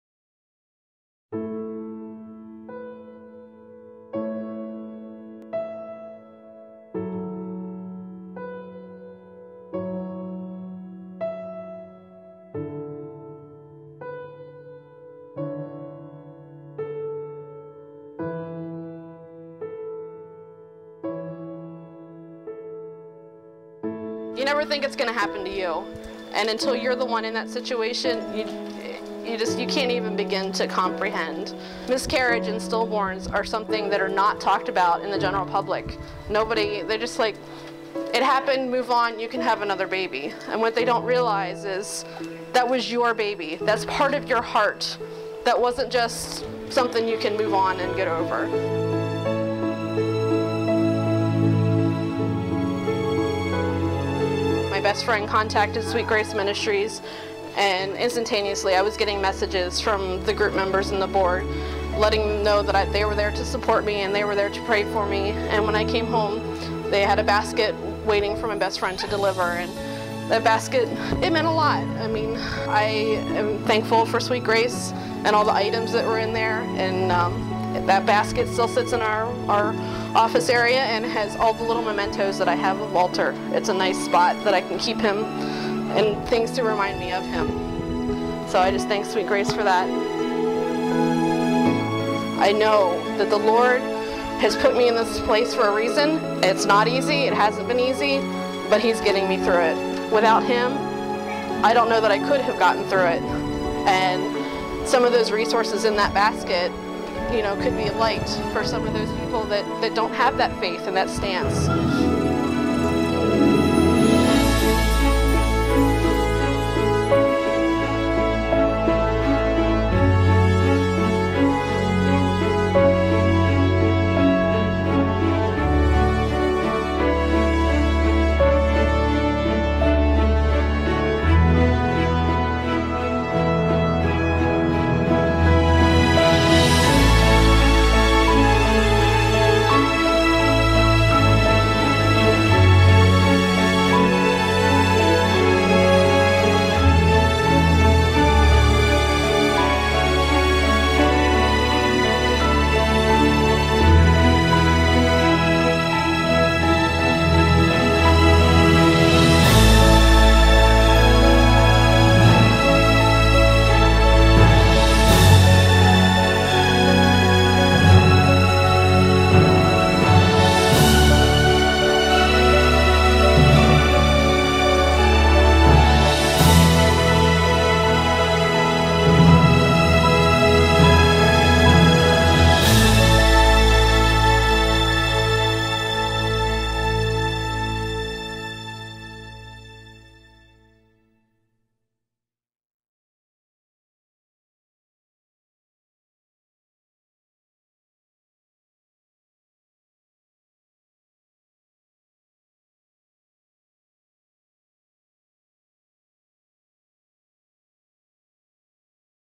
Sweet Grace Ministries: 2nd Annual Balloon Release // Recap
Recap of Sweet Grace Ministries' 2nd Annual Sweet Walk to Remember / Balloon Release held October 13, 2013 at Norlo Park in Chambersburg, PA.

Make sure to visit this wonderful organization to see how you can support those who have endured miscarriage, stillbirth or neo-natal death.